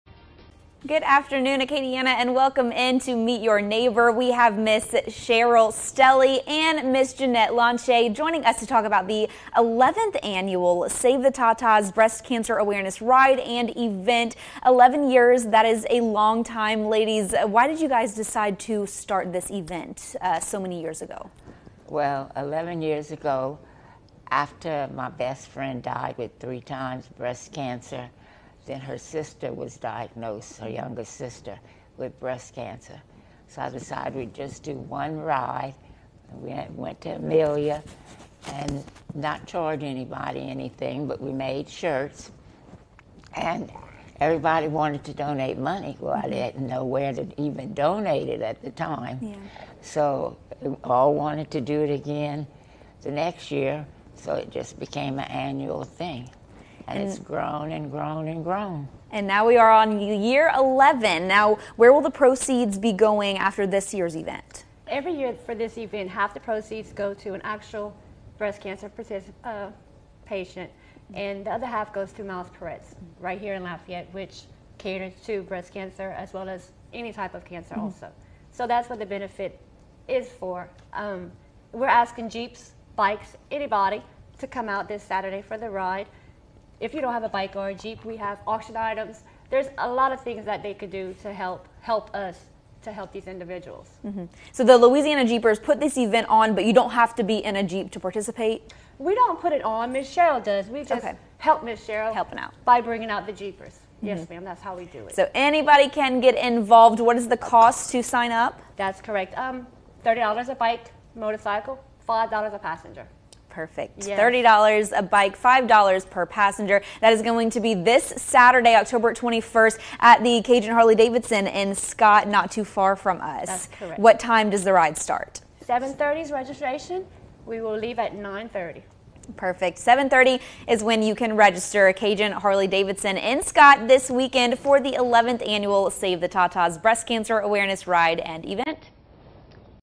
Meet Your Neighbor: Save the TaTa's
The 11th Annual Save the TaTa's Breast Cancer Awareness Ride is October 21 at Cajun Harley Davidson.